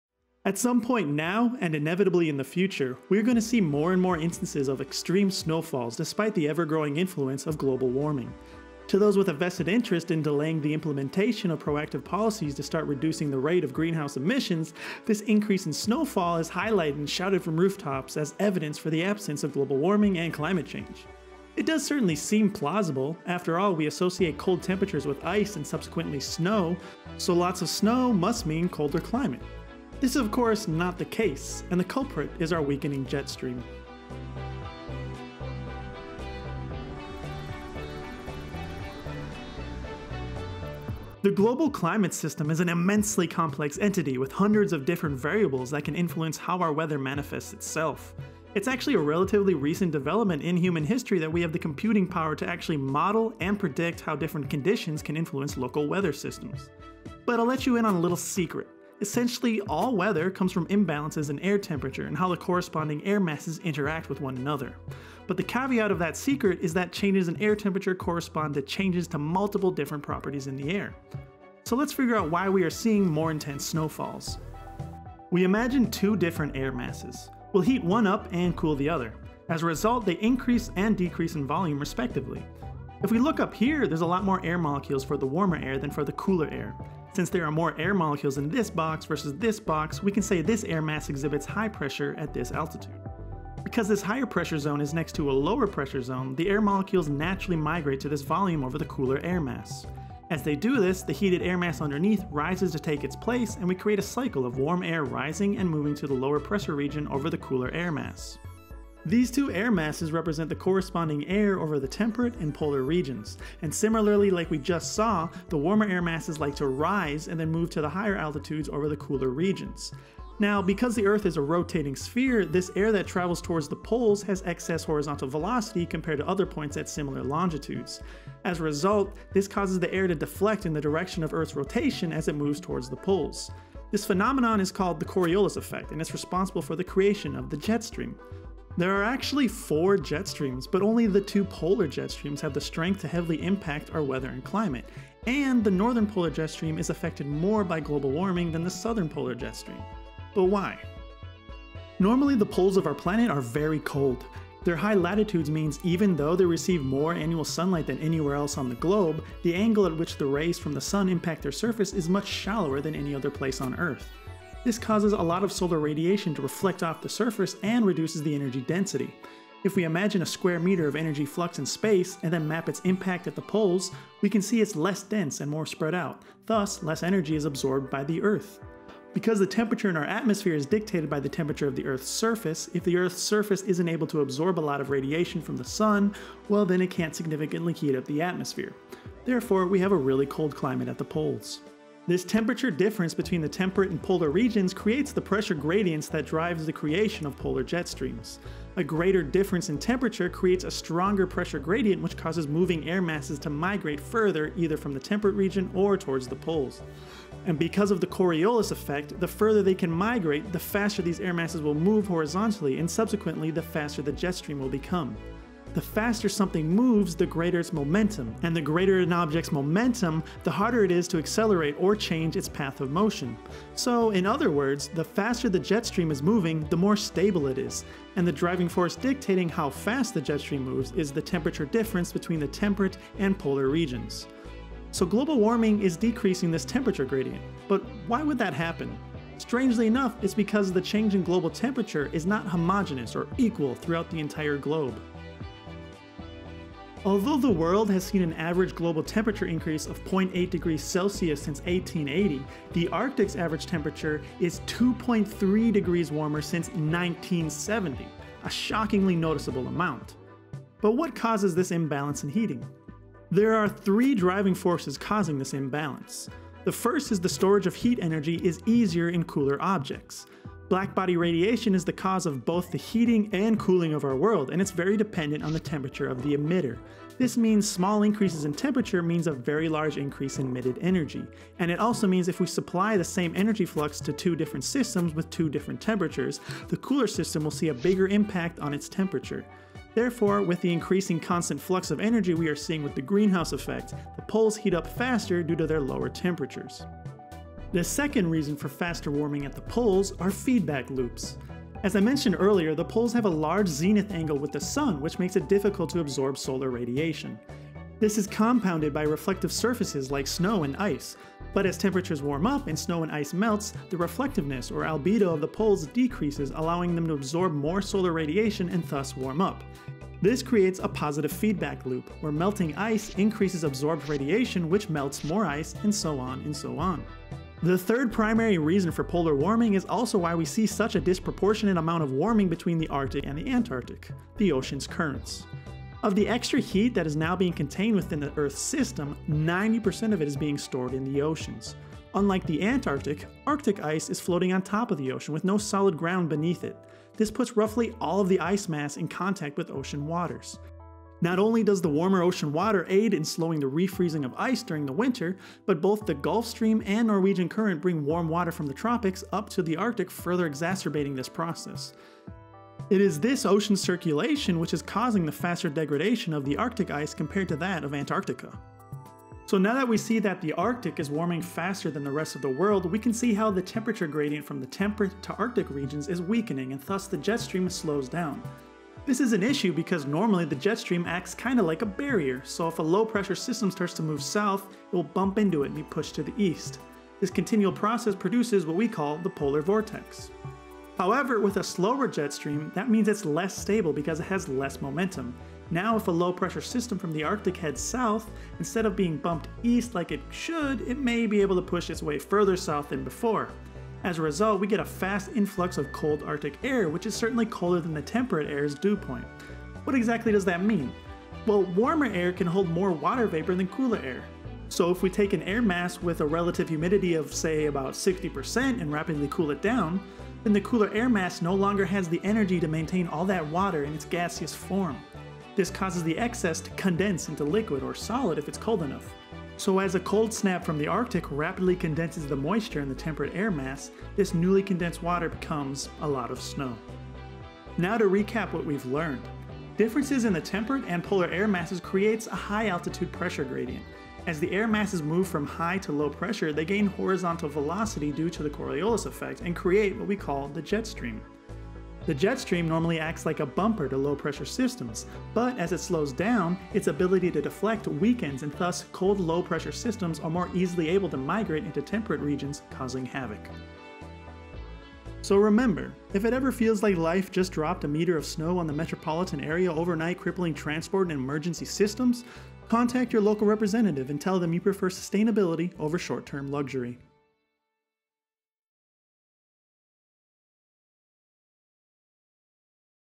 More Snow in a Warmer World
You may have noticed that the US seems to experience some kind of intense snow event almost annually now. Places that haven't seen snow in decades are starting to see white and places that are used to it are seeing a lot more in shorter periods of time.

In a world with an average temperature rapidly increasing, how can warmer global temperatures lead to more snowfall?